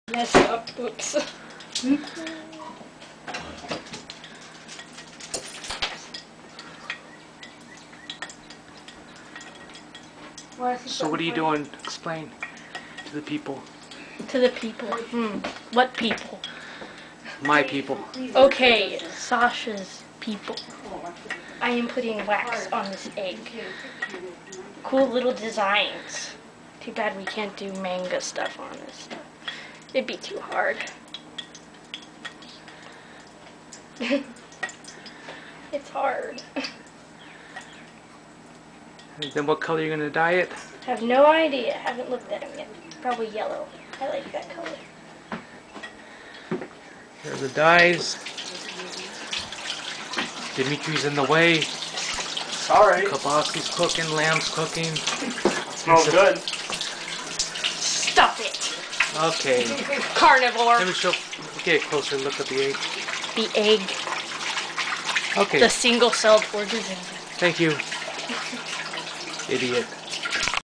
making Pascha eggs part 1
my niece making Pascha eggs in the Carpatho-Russian tradition